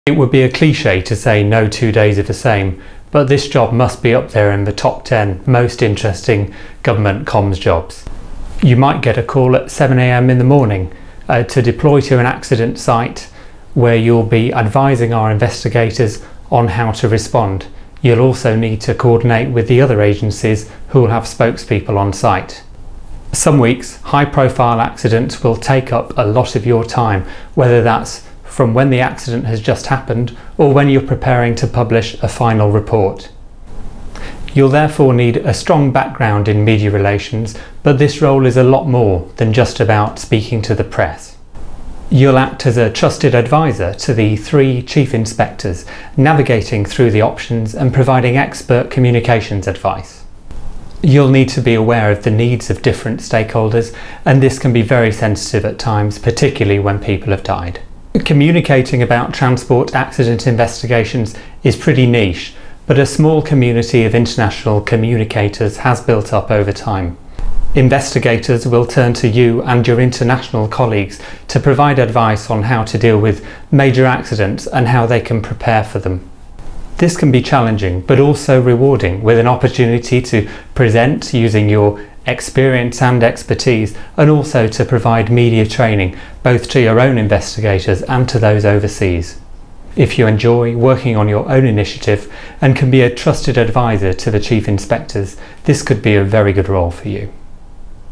Head of Communications for the Accident Investigation Branches
Our current Head of External Communications, James Hotson, talks about the role and providing expert strategic and tactical communications advice to the Air, Marine and Rail Accident Investigation Branches.

This video has subtitles